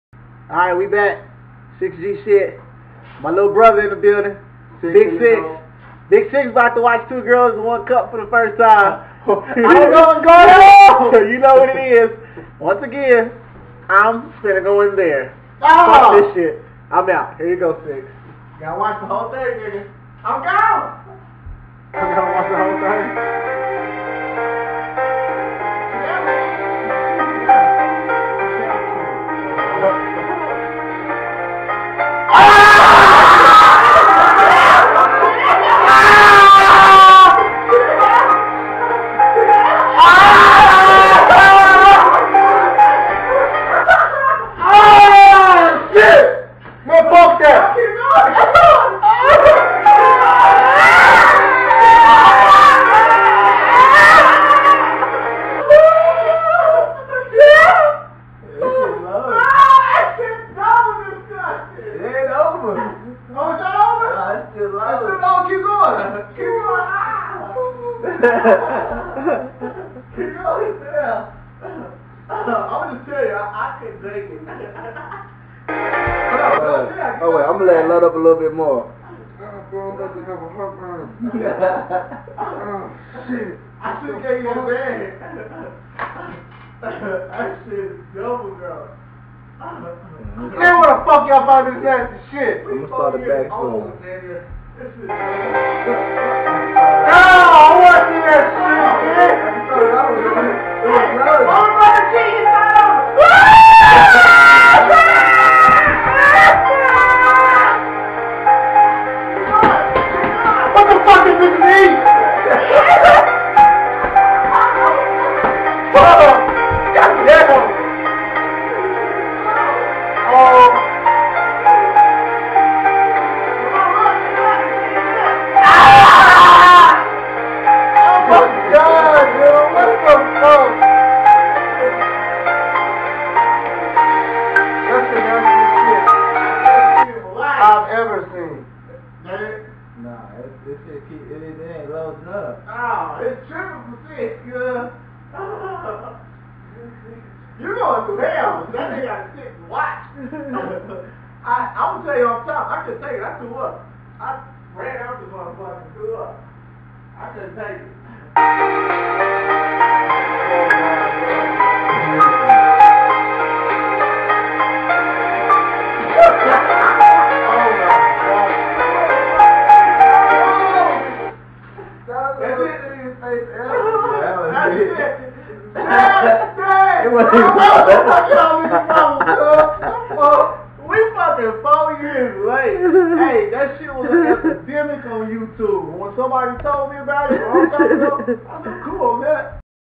2 girls 1 cup reaction.. LOL.. (Big 6)
Star & Super Jay makes Starr's lil brother Big 6 watch the 2 girls 1 cup video.. lmaoooo!!!

Instagram, Snapchat & Twitter - @LitMobStarr